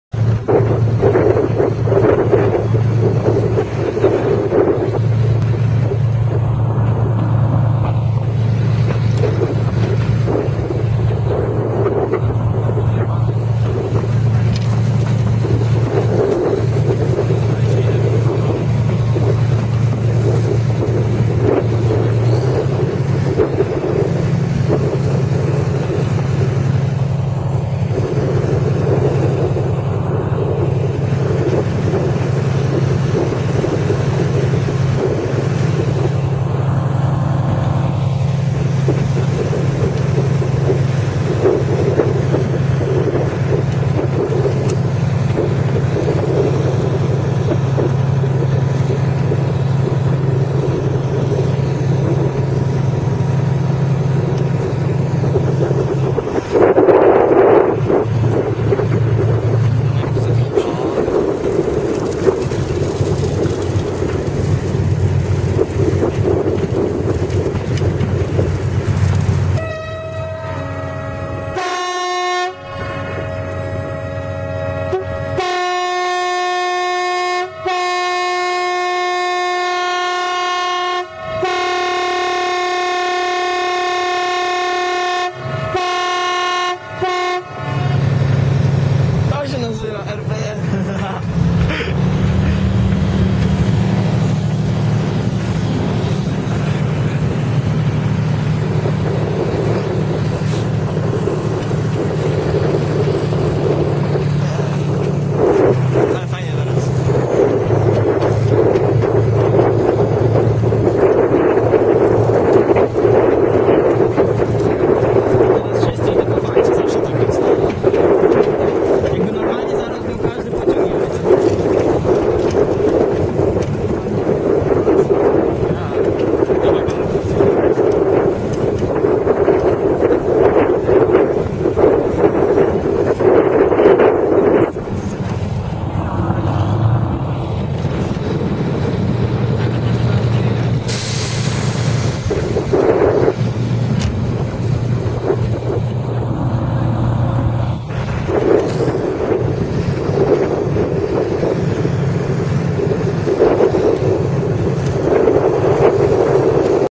Lxd2 334 i 268 z roboczym składem - przejazd przez e4
Czyli rumuny na podwójnej trakcji, Lxd2 334 był na testach a 268 był jako lokomotywa rezerwowa. Przejazd przez najniebezpieczniejszy przejazd na całej trasie, na drodze krajowej e-4, także można usłyszeć sławne 'mega rp1' :)
Pozdrowienia dla całej ekipy z tego dnia!